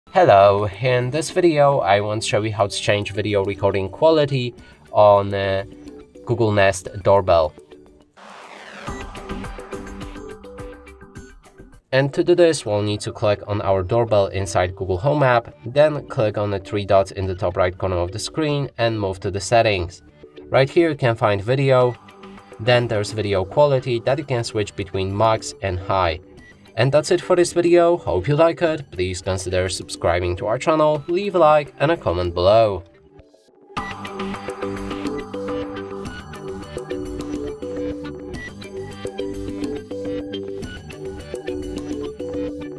How To Change Video Quality In Google Nest Doorbell
This instructional video provides a comprehensive guide on how to adjust the video quality settings in your Google Nest Doorbell. Whether you're looking to conserve bandwidth or enhance video clarity, understanding how to change the video quality ensures that your doorbell camera meets your specific needs. Follow along with the step-by-step instructions to customize the video quality settings according to your preferences.

Why might users want to change the video quality settings in their Google Nest Doorbell?
Can you explain the importance of adjusting video quality for optimal performance?
How do users access the settings menu for their Google Nest Doorbell within the Nest app?
Where specifically can users find the option to change video quality settings?
What are the available options for video quality settings, and what do they signify?
How does changing the video quality setting affect bandwidth usage and video clarity?